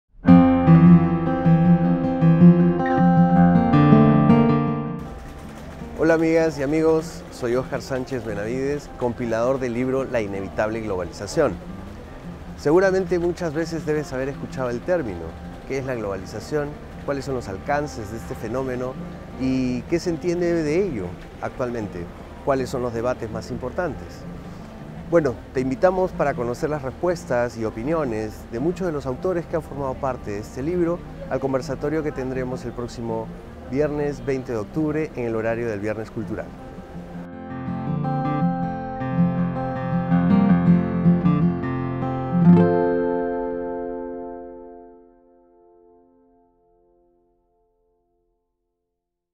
Conversatorio con autores del libro "La Inevitable Globalización"
En el conversatorio con autores del libro "La Inevitable Globalización: enfoque cultural y económico del escenario mundial" buscan compartir y someter a discusión con los estudiantes y el público en general, las reflexiones que sobre el tema han realizado algunos profesores en el marco de su labor docente.

El libro está compuesto por ocho artículos de divulgación en los que se mantiene una pauta general: cada autor ha seleccionado un tema de interés y lo analiza teniendo en cuenta los referentes académicos más importantes de la globalización contemporánea.